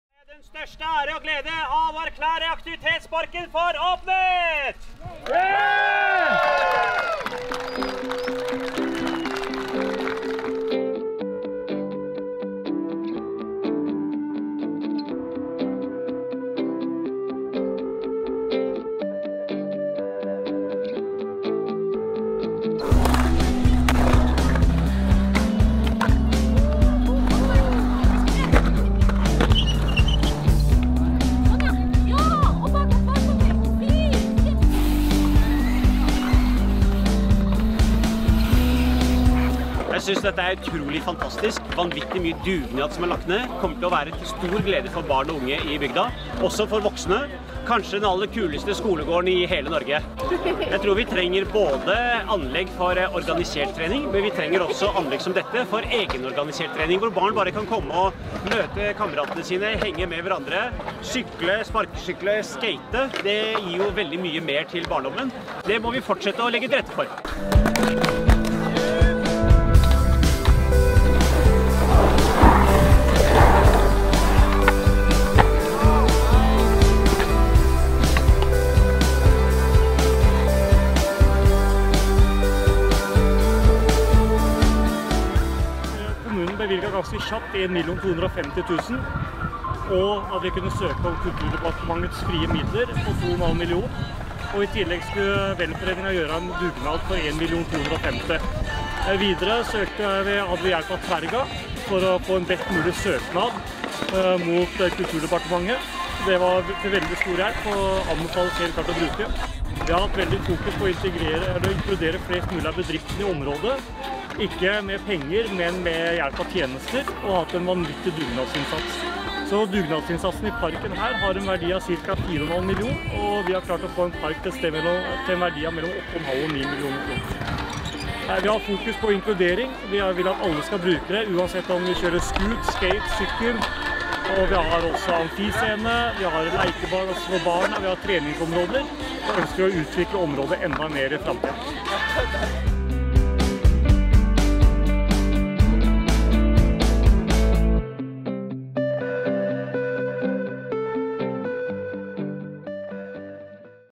Åpning av Kryllingparken på Krøderen
Kryllingparken er en aktivitetspark bygget og designet av Betongpark.

Skate og - aktivitetspark med skatepark og pumptrack. Forbildeanlegg av Gode Idrettsanlegg.

Aktivitetsparken skal sto ferdig i 2020, og er et sted for alle aldre og evner, og fungerer som et helårsåpent tilfluktssted for trenings- og ekstremsportentusiaster i lokalområdet. Parken inneholder en skatepark, pumpebane, parkour -område, hangouts, dansescene og jibbakke for vintersport.

Hovedaktiviteten i aktivitetsparkene er en hybrid-skatepark som kombinerer flere disipliner til en morsom, tilgjengelig og brukervennlig skatepark. Parken består av to kombinerte seksjoner og har en åpen bowl, miniramp - seksjon og utvidet bowlhjørne, samt to hips. Den andre delen er en forhøyet Street Plaza som inneholder mange streer elementer, bl.a rails, trapper, banks, ledges og wallride. Samt en stor pumptrack i asfalt.  En ekstra miniramp i tre er også finne der.